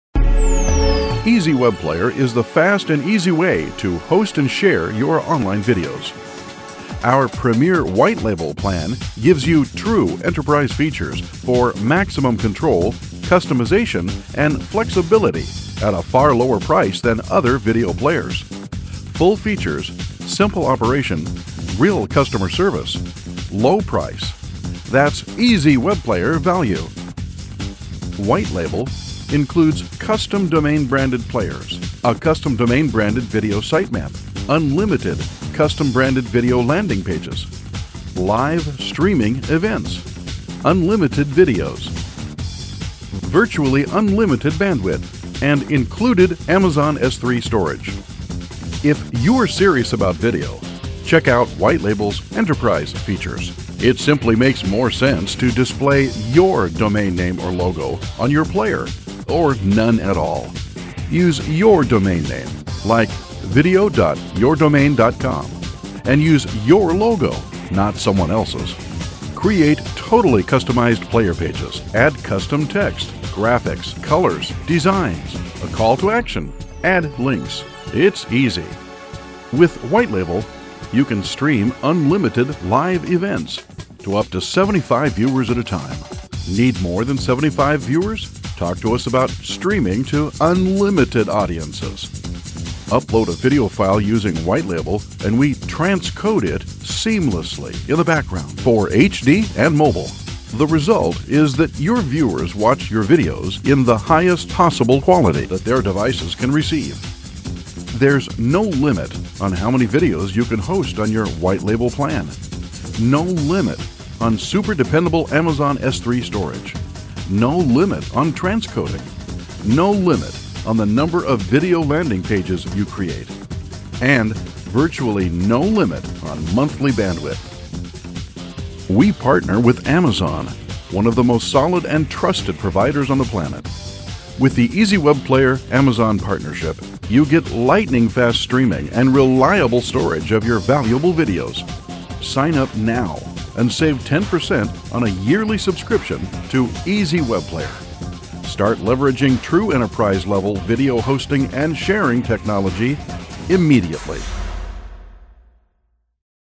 EZWebPlayer's WHITE LABEL - Domain Branding; HD & Mobile Playback; Custom Landing Pages; much more!
This video introduces the web's premium video hosting and sharing package: EZWebPlayer's White Label. It's packed with premier, enterprise-level features. And it's easy for any business to use — if you can copy and paste you can manage your video library like a pro! Features include Advertising Call-to-Actions; Video Sitemap; Unlimited Custom Video Landing Pages; Custom Web Domain Branding; Unlimited Videos; and more.